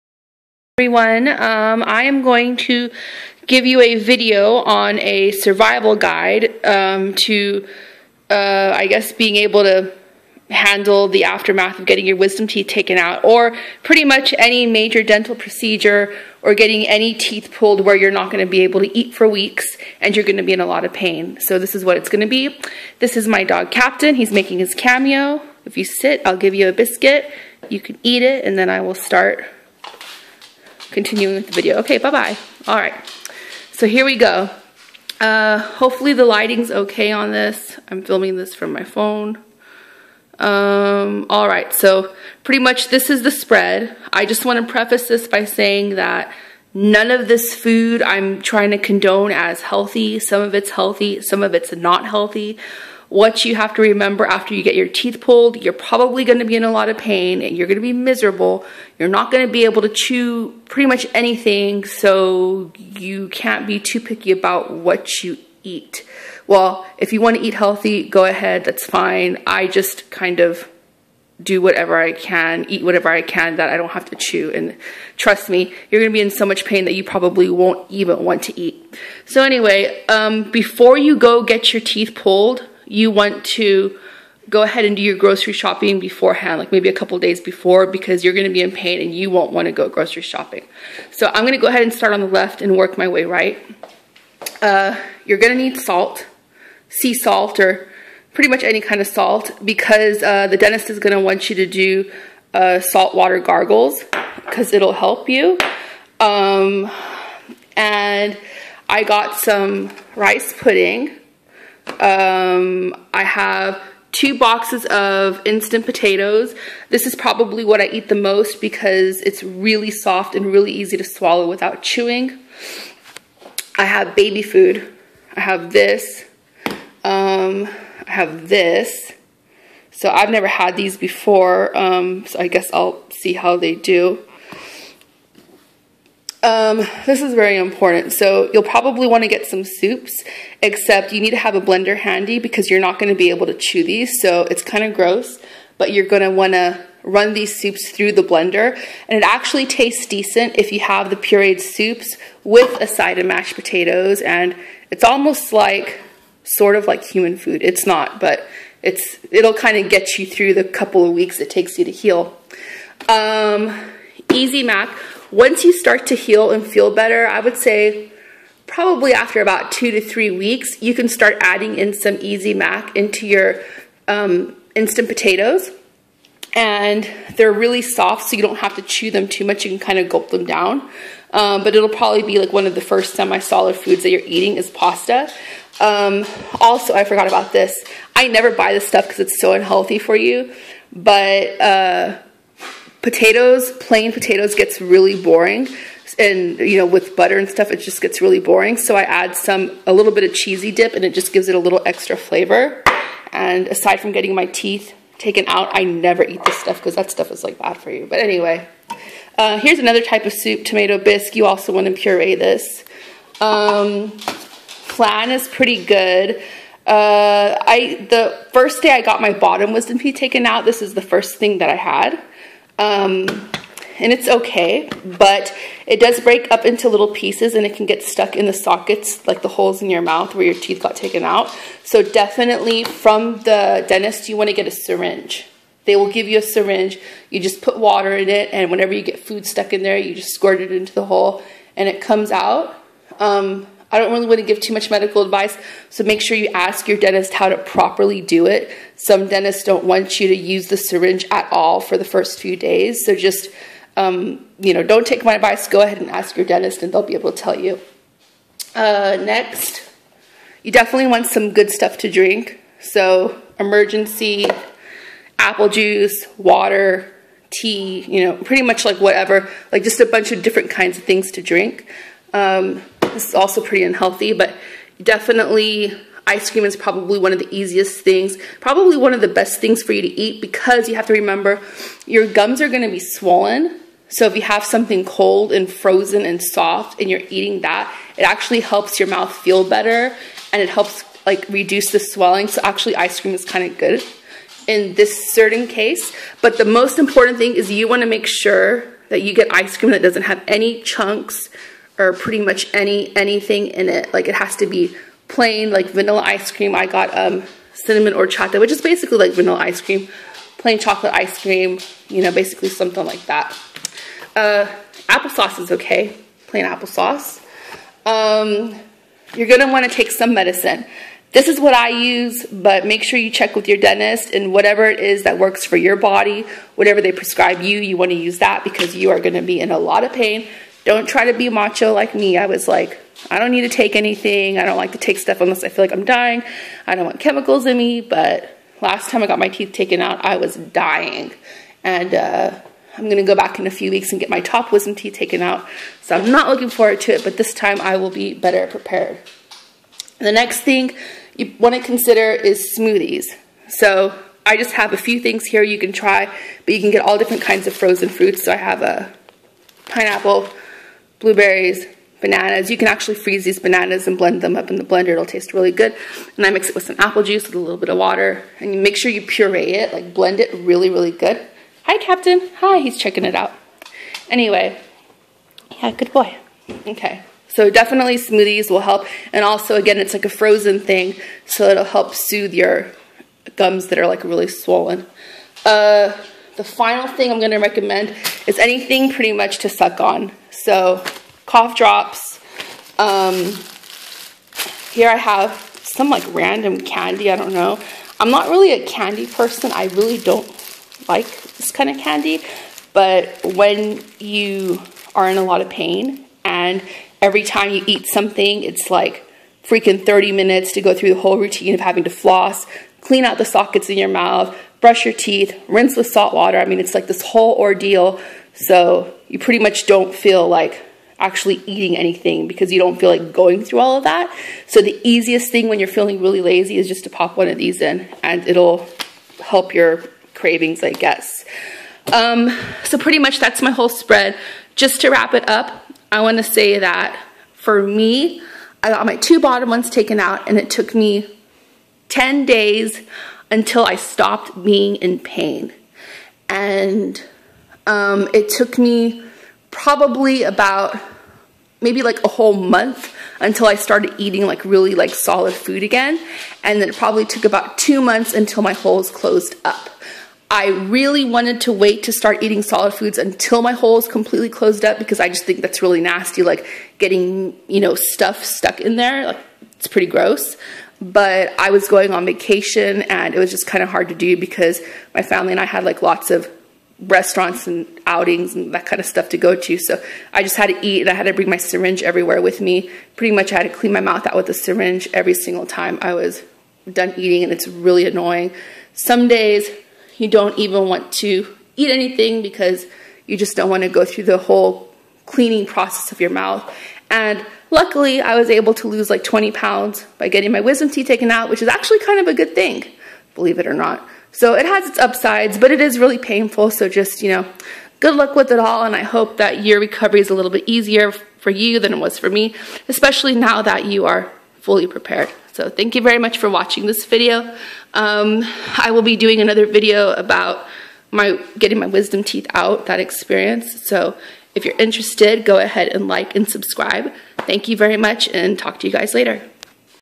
Wisdom Tooth Survival Guide
WATCH MY WISDOM TOOTH EXPERIENCE (I VLOG EVERYDAY FOR 7 DAYS AFTER MY TOOTH EXTRACTION)

Wondering what to eat and what to do after you get your teeth pulled? Watch this! Be prepared to have your wisdom tooth extracted.